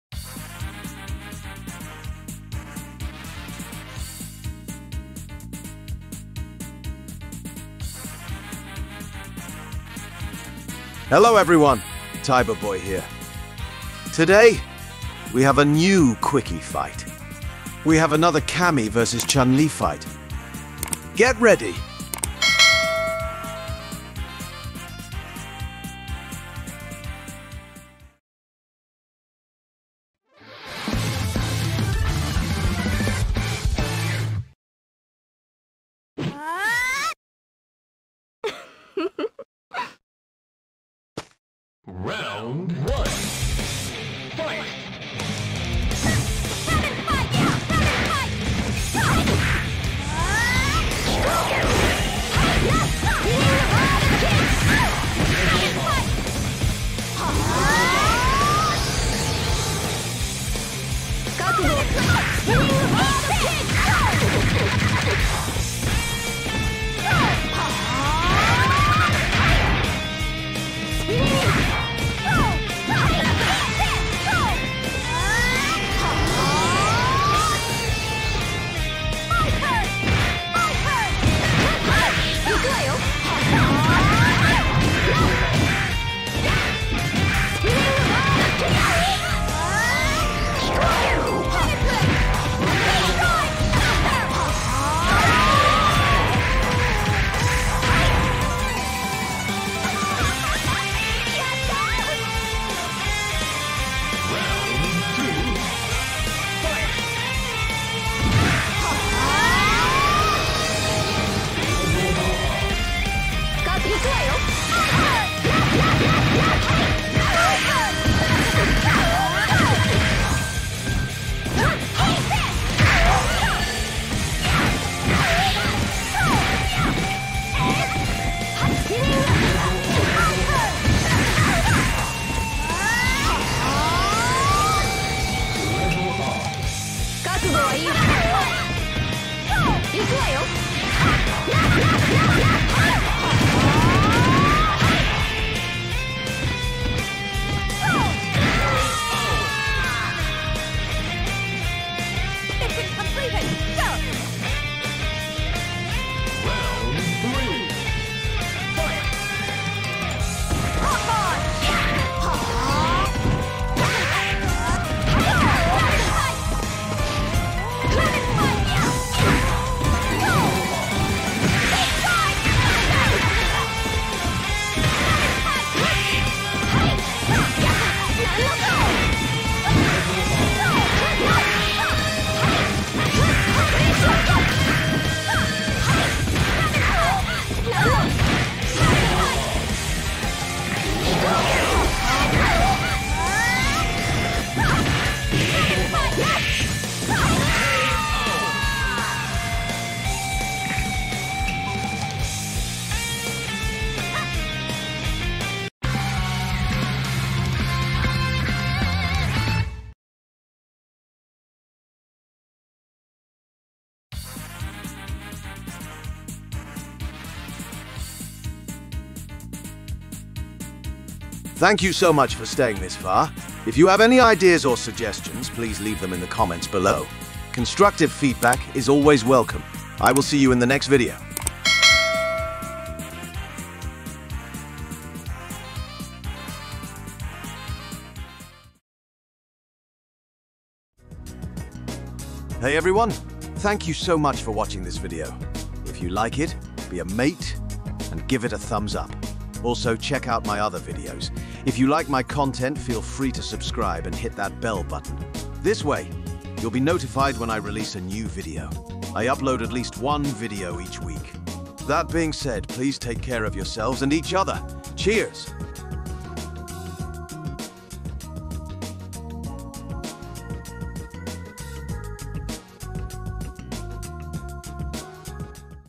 [QUICK-ie FIGHTS] #049: Cammy Vs Chun-Li: THE STRONGEST WOMAN IN THE WORLD: PART 4
*Quick-ie Fight 0049*

If you can't see this video, try to see it on My Rumble Channel:

Cammy Vs Chun-Li
Type of Fight: 1-on-1 Fights, Best 2 out of 3.
System: IKEMEN-Go

*CHAPTERS*
0:09 Intro
0:29 Cammy Vs Chun-Li
3:33 Ending
4:00 Outro

*CREDITS*
Cammy created by "Chuchoryu & Varo_Hades" / Edited by Tiger-Boy.
Chun-Li cretaed by "Jmorphman".

*Stages*
-"GeeseTower2014"
-Created by Shiyo Kakuge / Edited by Agh07.

Constructive Feedback is always Welcome.
If you have any advise or ideas,
or if there are any issues, please let me know.

*[~TB~]*